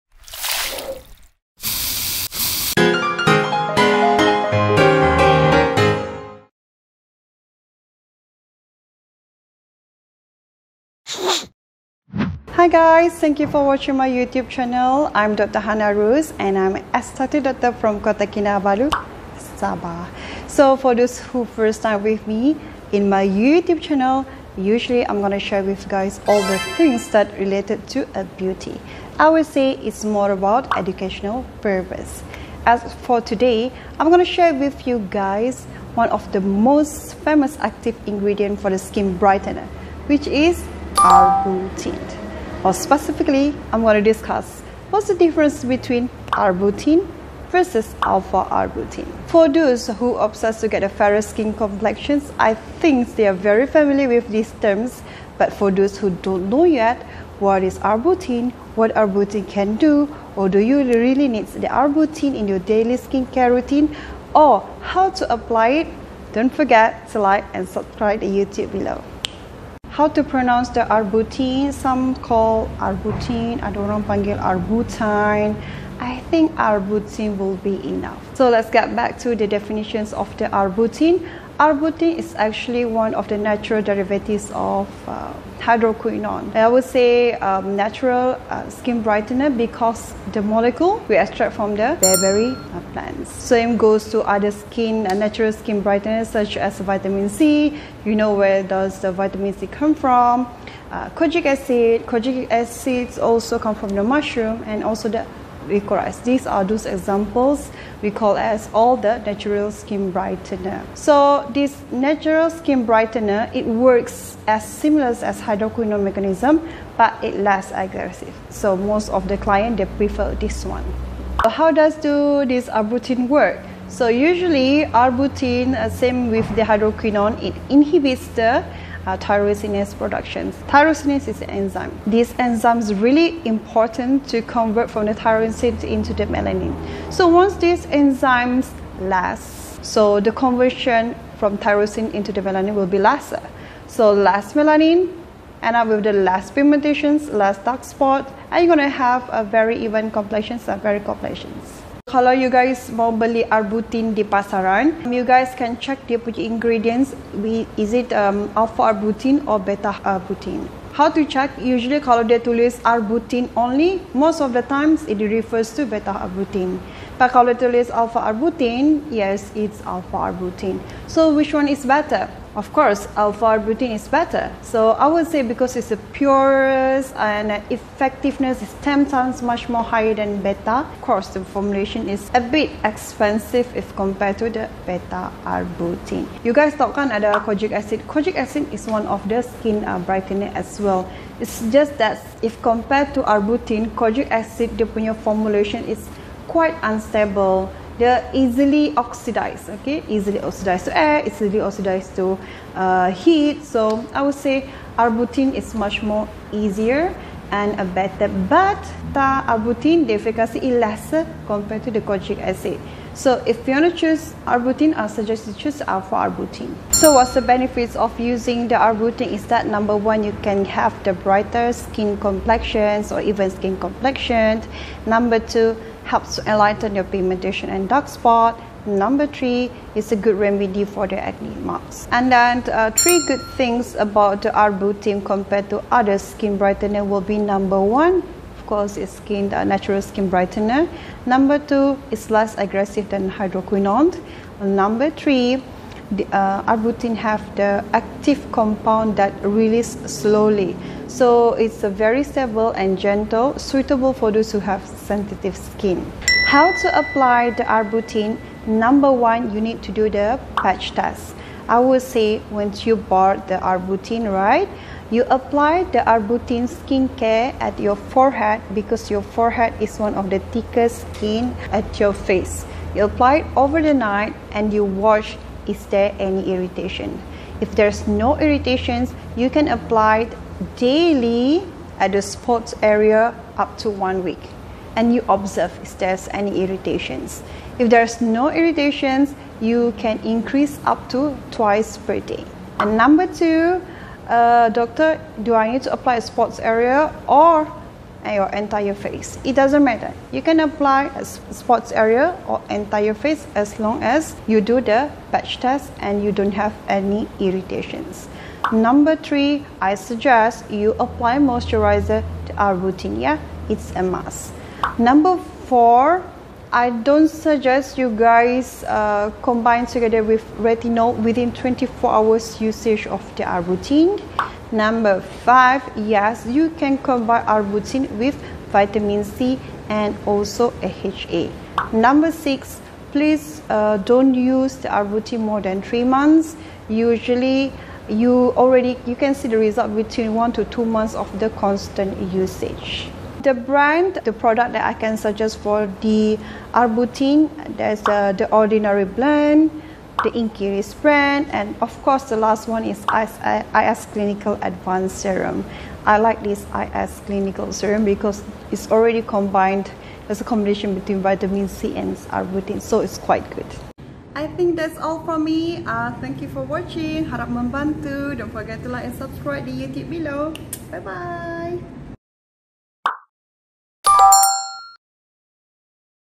‘Glow Up With Dr Hana’ - Topic: The Wonders of Arbutin to Your Skin
The most common ingredient in skin brightener is Arbutin. In this episodes, Dr Hana shares the differences between Alpha Arbutin and Beta Arbutin. And how you can decide on your next skin brightener.